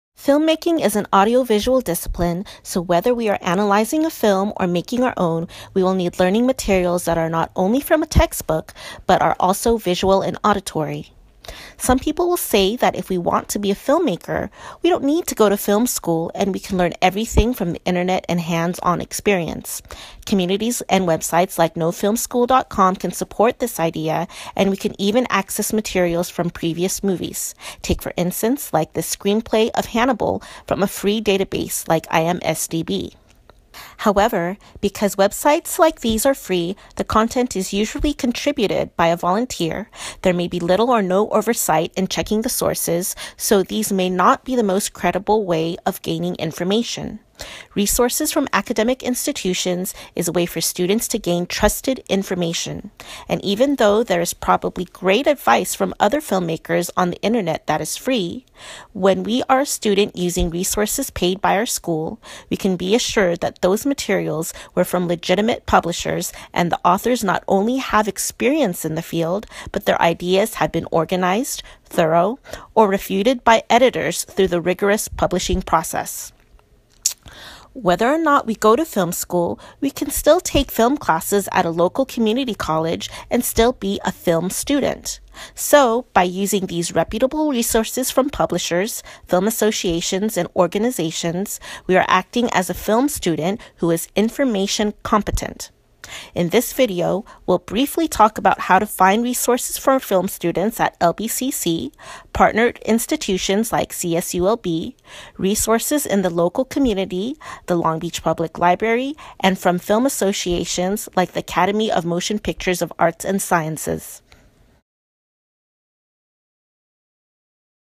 Resources for Film Students 1/6 - Introduction: Free Information Vs. Paid Information
LBCC - Information Competency for Film Students
Special Thanks to Librarians Colin, Gail and Judith at LBCC

Filmmaking is an audiovisual discipline, so whether we are analyzing a film or making our own, we will need learning materials that are not only from a textbook but are also visual and auditory. Some people will say that if we want to be a filmmaker, we don’t need to go to film school and we can learn everything from the internet and hands-on experience. Communities and websites like NoFilmSchool.com can support this idea and we can even access materials from previous movies. Take, for instance, like this screenplay of Hannibal, from a free databases like imsdb.

However, because websites like these are free, the content is usually contributed by a volunteer,   Resources from academic institutions is a way for students gain trusted information. And, even though there is probably great advice from other filmmakers on the internet that is free, when we are a student using resources paid by our school, we can be assured that those materials were from legitimate publishers and the authors not only have the experience in the field, but their ideas have been organized, thorough, or refuted by editors through the rigorous publishing process.

Whether or not we go to film school, we can still take film classes at a local community college and still be a film student. So, by using these reputable resources from publishers, film associations and organizations, we are acting as a film student who is information-competent. [insert pics of Speilberg]
In this video, we’ll, briefly, talk about how to find resources for film students at LBCC, partnered institutions like CSULB, resources in the local community: the Long Beach Public Library and
from Film Associations like the Academy of Motion Pictures of Arts and Sciences